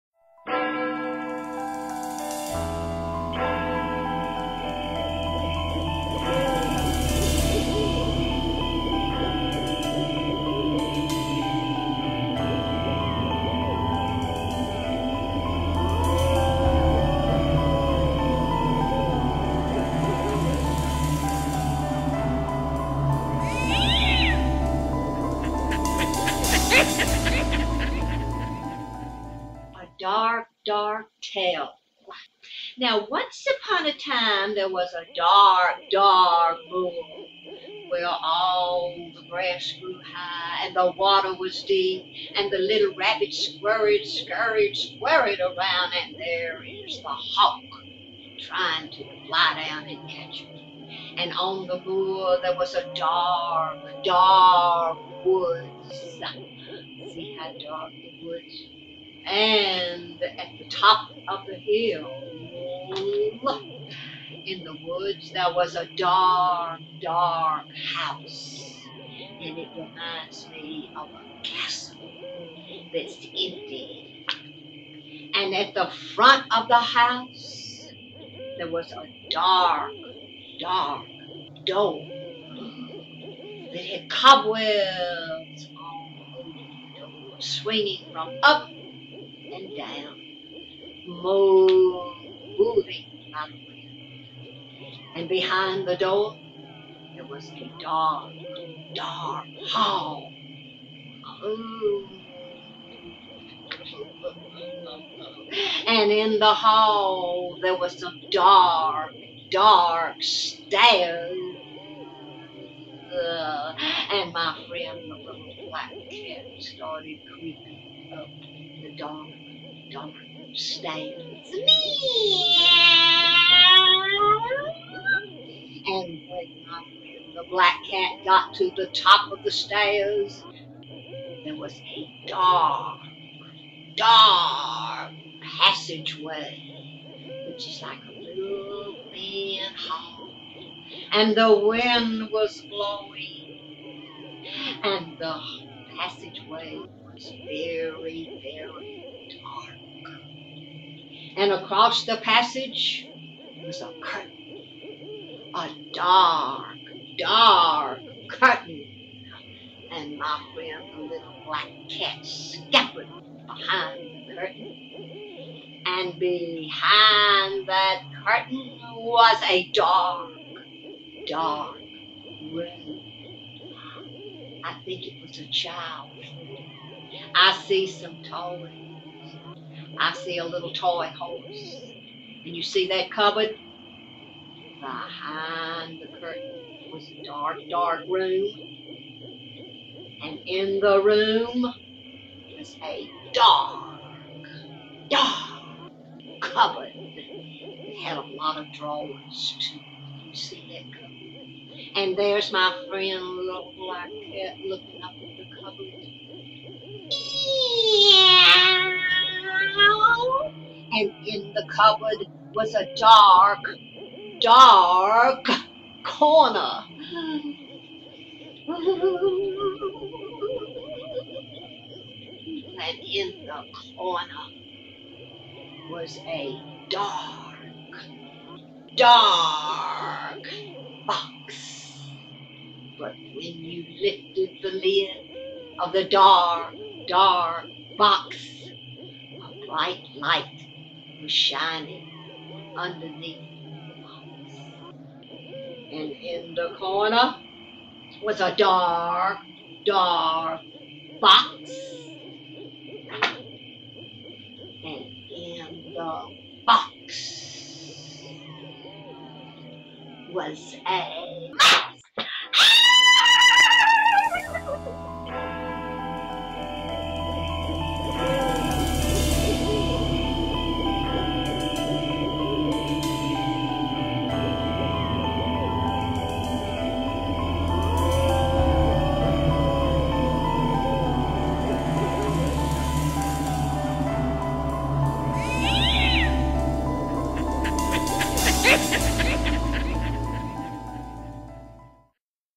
Storytime with Mother Goose: "A Dark Dark Tale"
Mother Goose reads "A Dark Dark Tale" by Ruth Brown for Halloween.  Mother goose reads every week.  for more information.